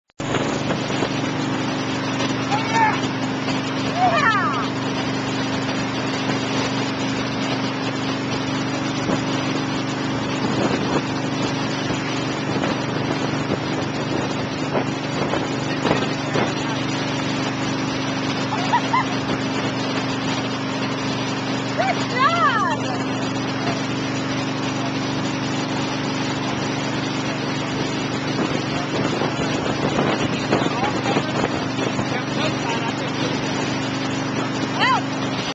5 year old wakeboarder clears wake!!!
Our 5 year old son clearing the wake for the first time!!